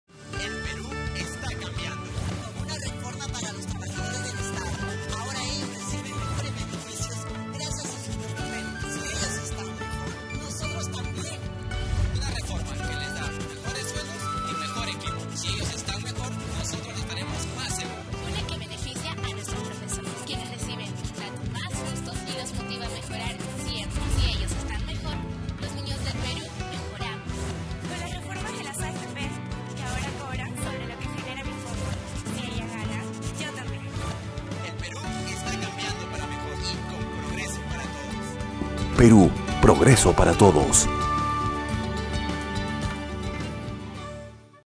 Spot Reformas: El Perú esta cambiando - 1
Reformas de la Policía y las Fuerzas Armadas, Ley del Servicio Civil, Reforma Magisterial y Reforma del Sistema Privado de Pensiones.